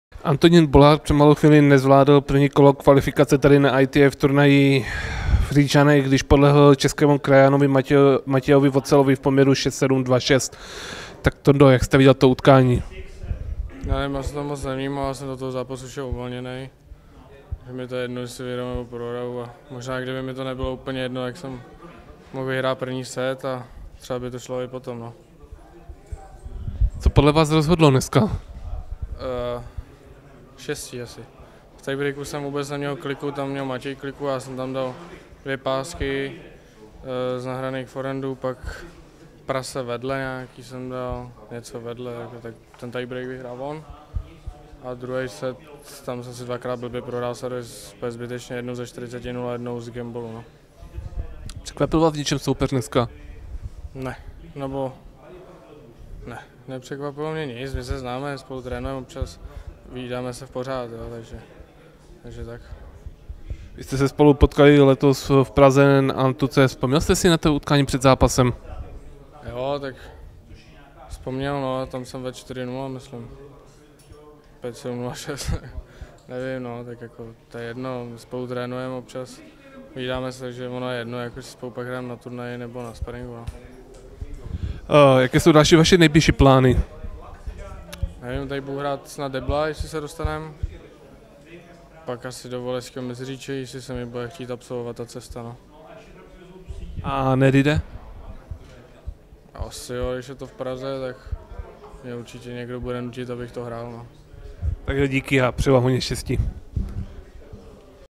Antonín Bolardt po porážce v 1. kole kvalifikace ŘÍČANY INDOOR BY PENTA TRADING
1. kolo kvalifikace ŘÍČANY INDOOR BY PENTA TRADING: MATĚJ VOCEL (986.) – ANTONÍN BOLARDT (1395.) 7:6 (3), 6:3